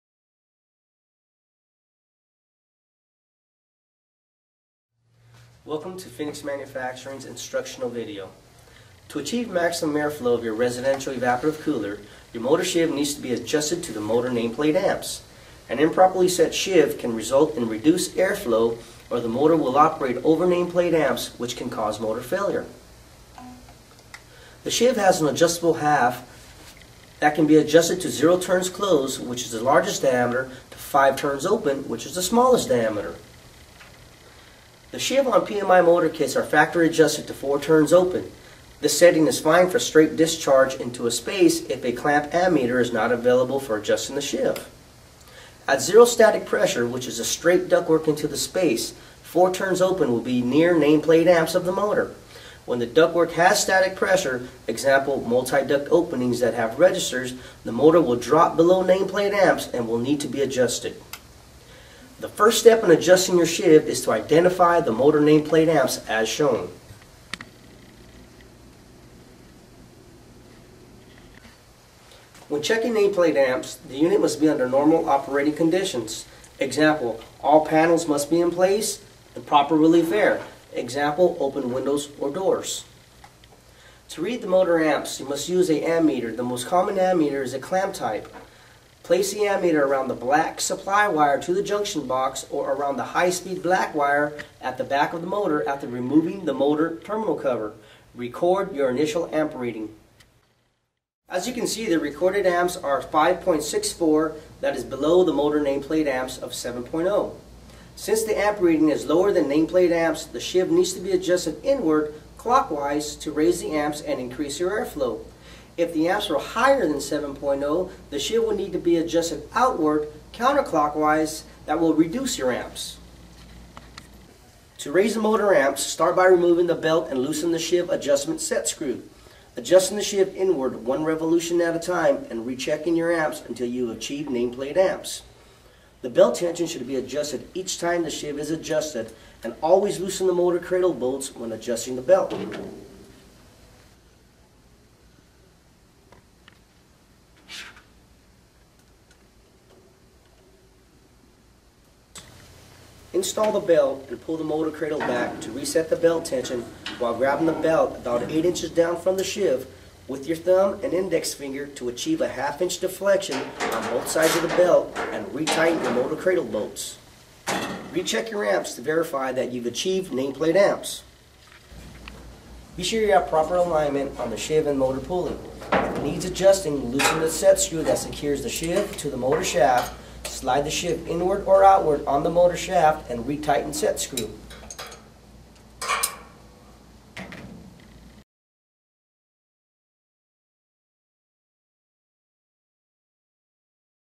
How to Adjust the Motor Sheave on a PMI Residential Cooler
Watch an overview of how to adjust the Motor Sheave on a Phoenix Manufacturing Residential Evaporation Air Cooler. A properly adjusted Motor Sheave provides maximum air flow volume and allows the cooler to operate within the correct motor Amps range to ensure years of reliable service.